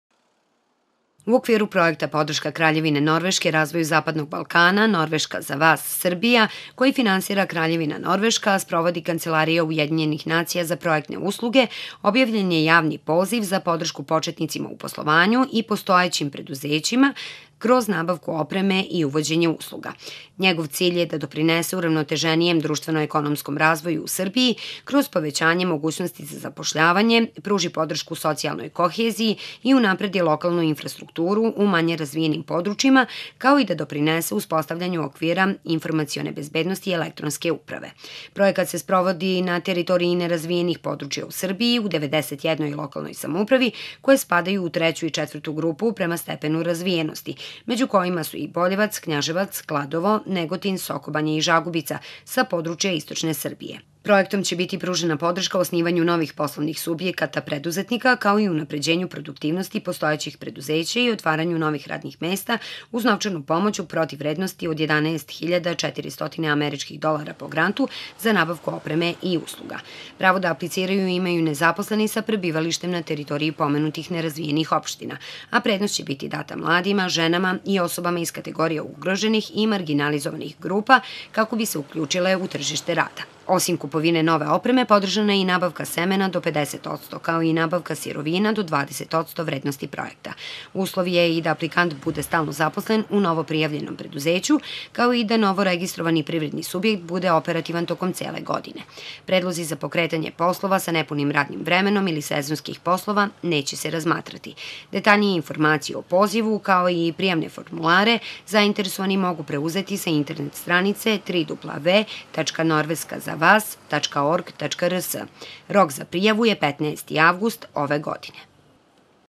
Norveska za vas, 11.avgust 2019. (RTV Bor)
U okviru projekta ''Podrška Kraljevine Norveške razvoju Zapadnog Balkana - Norveška za vas – Srbija“, koji finansira Kraljevina Norveška, a sprovodi Kancelarija Ujedinjenih nacija za projektne usluge, objavljen je javni poziv za podršku početnicima u poslovanju i postojećim preduzećima za razvoj, kroz nabavku opreme i uvođenje usluga. Pravo da apliciraju imaju nezaposleni sa teritoriji 91 nerazvijene opštine u zemlji, među kojima je i šest sa područja istočne Srbije.